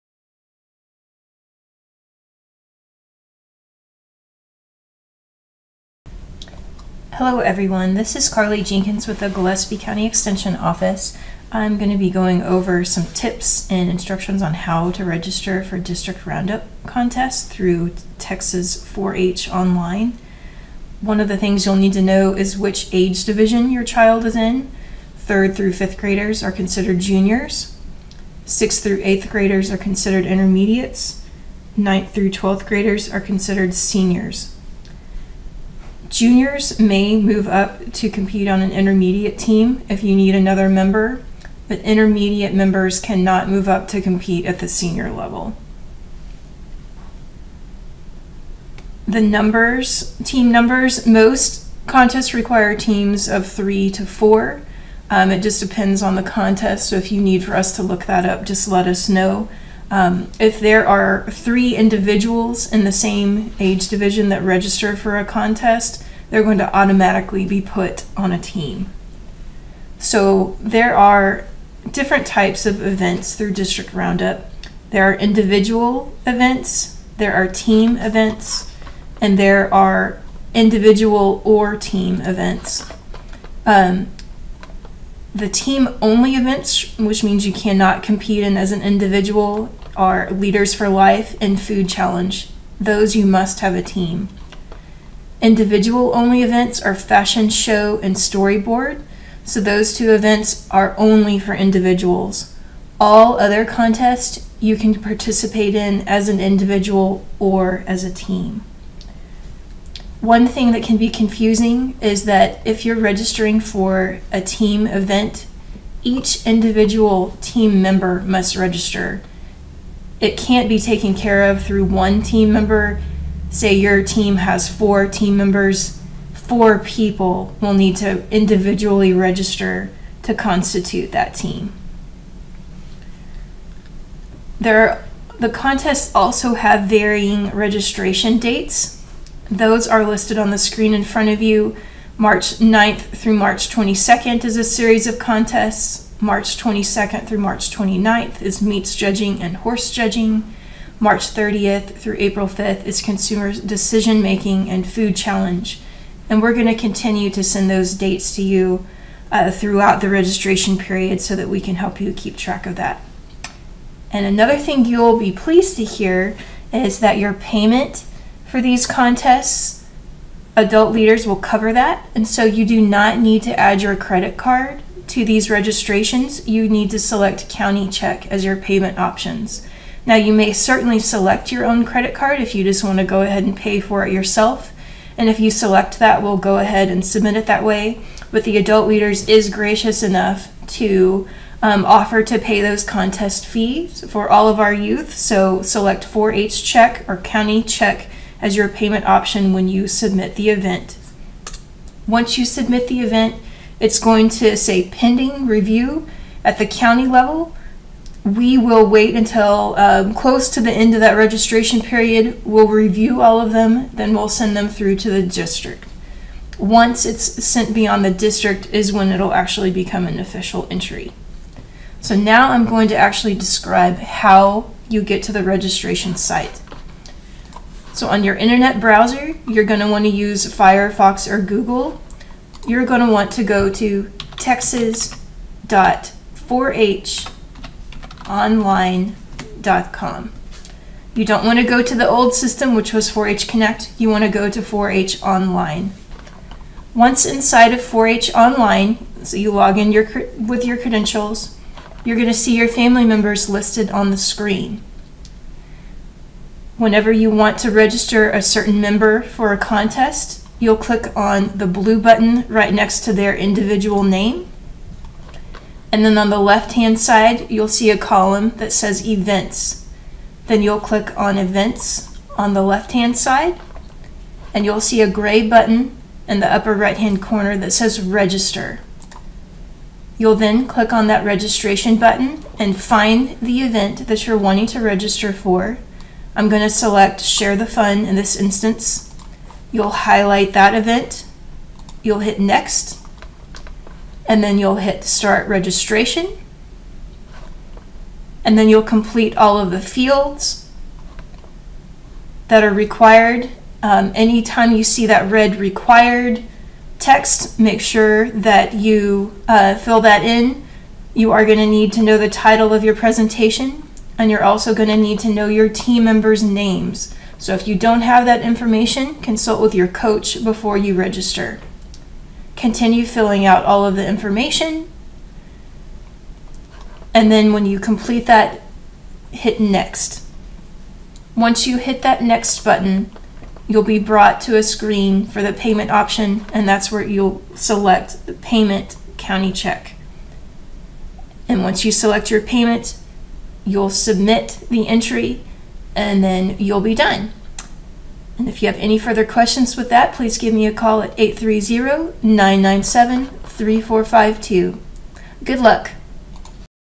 How To Register For Spring 2021 District Roundup Contests
This video will walk you through the process for registering for a Spring District 4-H Roundup Contest in Gillespie County.